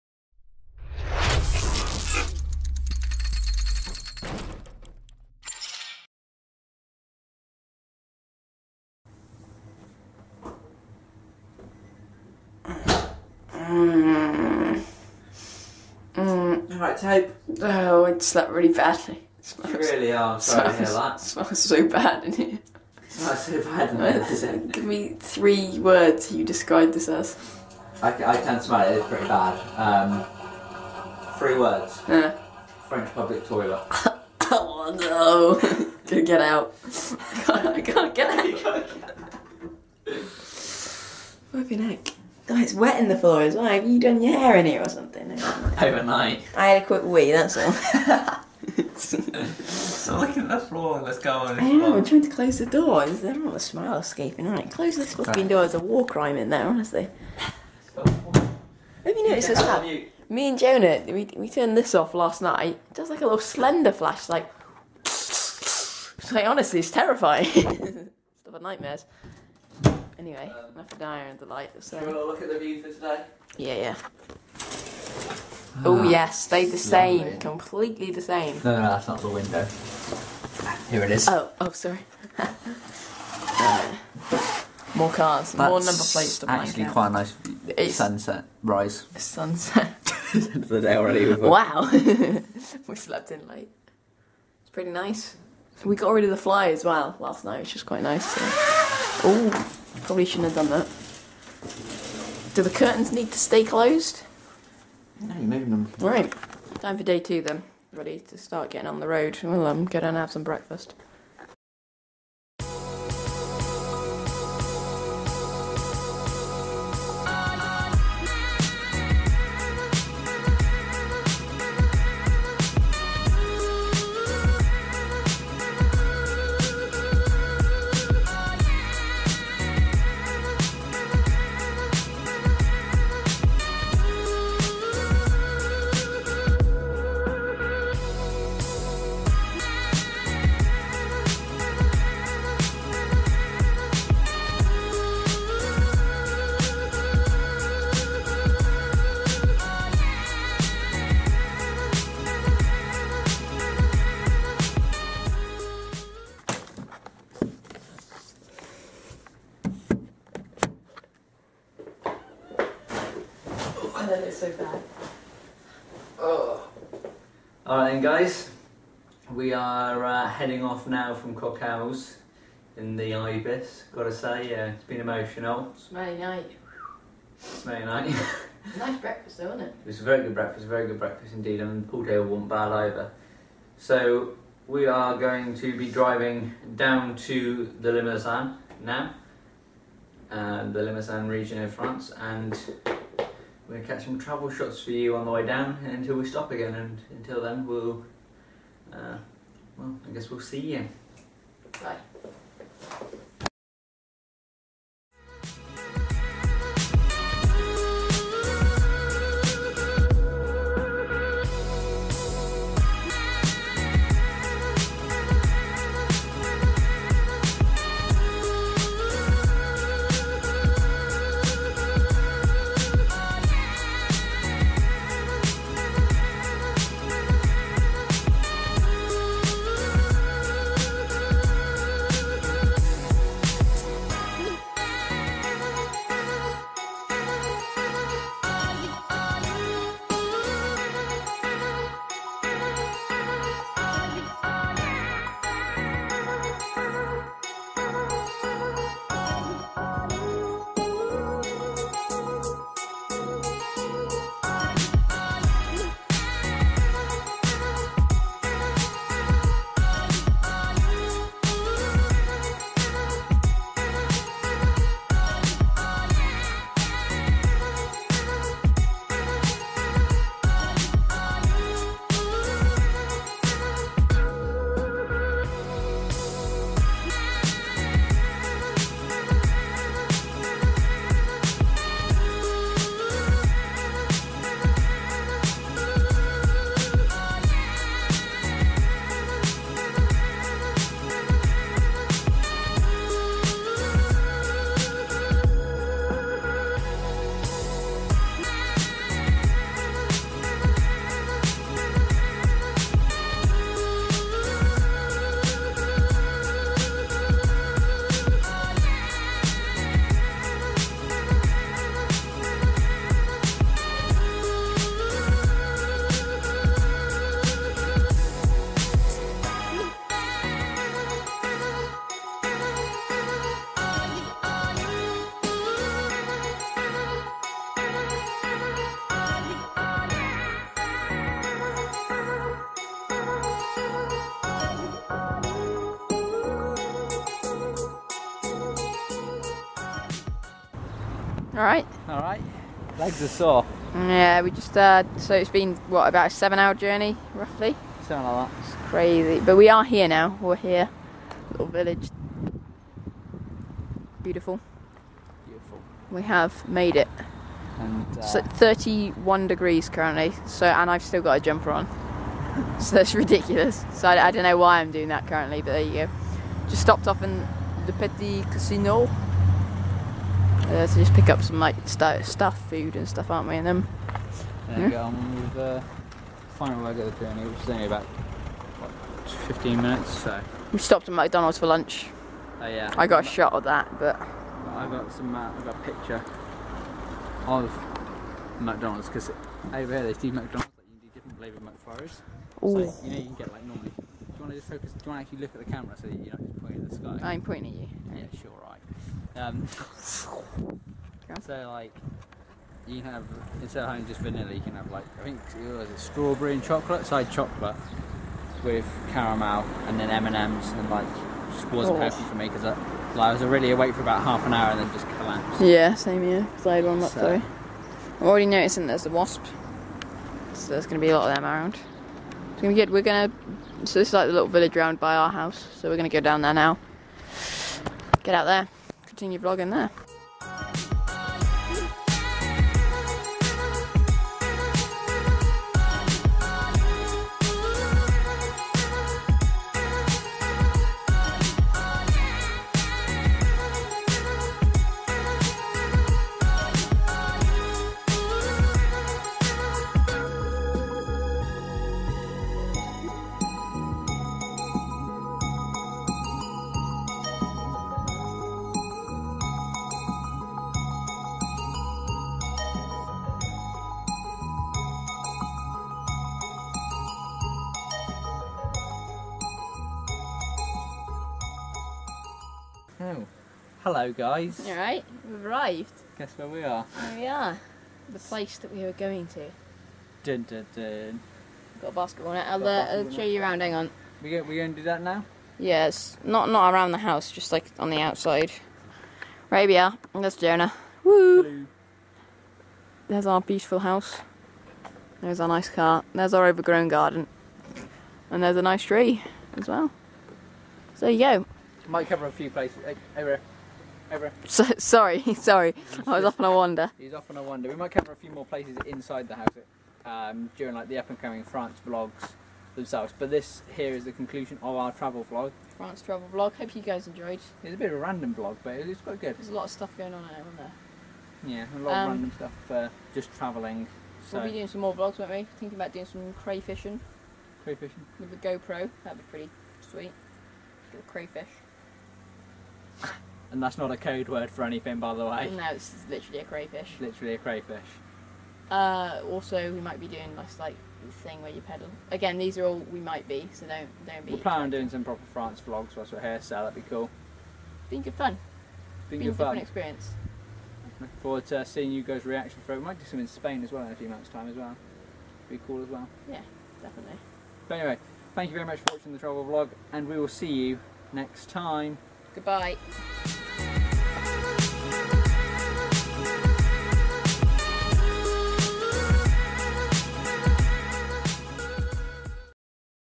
France travel vlog- part 2
The second video in our travel vlog see's us do a lot of driving and see some of the scenes around the French countryside.